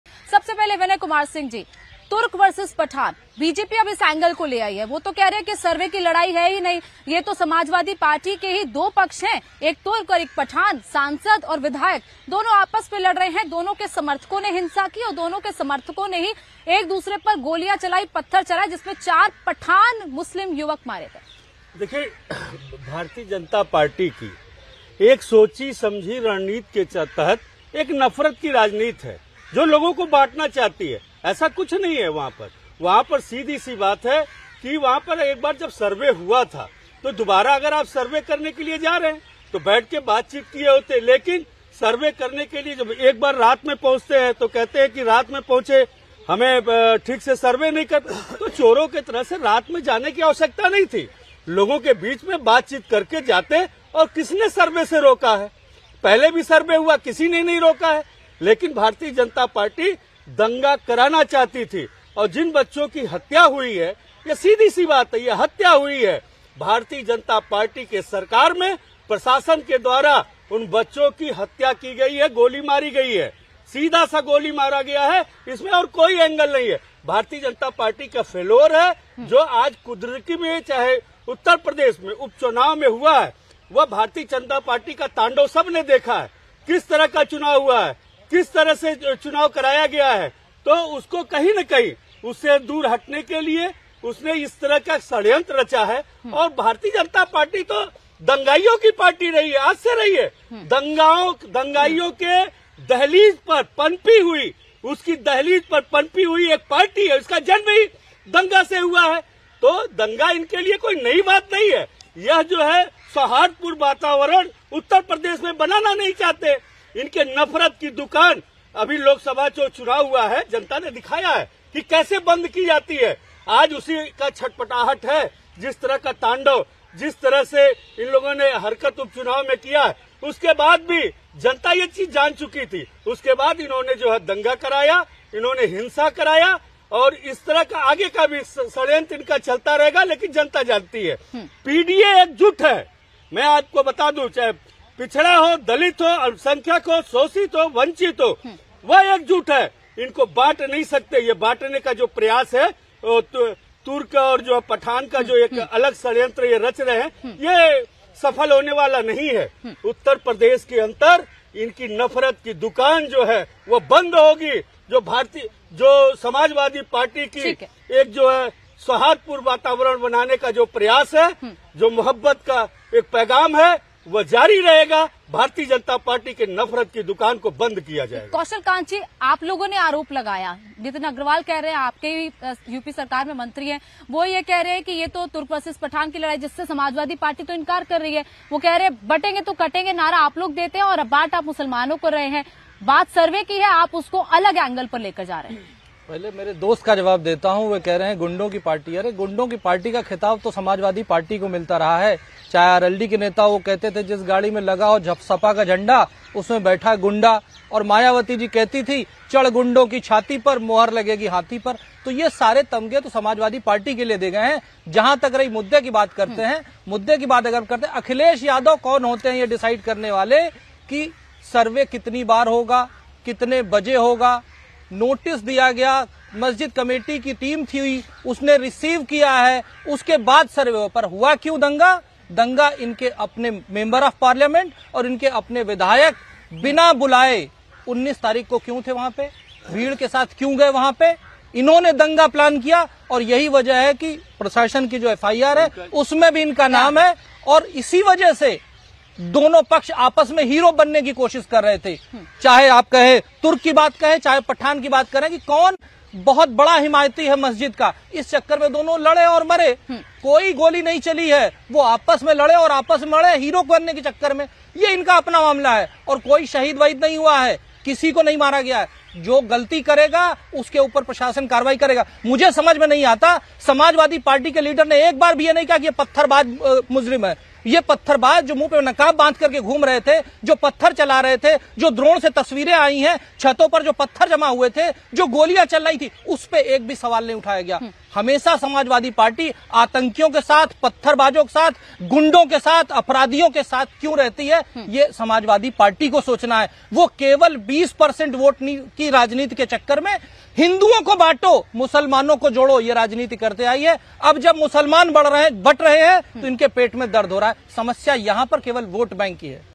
Sambhal Violence Update : क्या यूपी में तुर्क मुसलमान और पठान टकराए? | Muqabala | Jama Masjid Survey
उत्तर प्रदेश के संभल में दो दिन पहले भारी बवाल हुआ था। संभल मस्जिद का सर्वे करने गई टीम पर हमला हुआ था. आगजनी और तोड़फोड़ हुई थी, गोलियां तक चली थी जिसमें 4 मुस्लिम लड़कों की मौत हो गई। पहले तो ये लगा कि सर्वे को लेकर बवाल हुआ है ..गोली चली है और चार लड़कों की जान चली गई. लेकिन अब जब जांच आगे बढ़ी तो, हिंसा को लेकर बहुत बड़ा खुलासा हुआ.

There was a huge uproar in Sambhal, Uttar Pradesh two days ago. The team that went to survey the Sambhal mosque was attacked. There was arson and vandalism, and even firing took place in which four Muslim boys died. At first it seemed that there was an uproar over the survey… bullets were fired and four boys lost their lives. But now when the investigation progressed, a big revelation came out about the violence.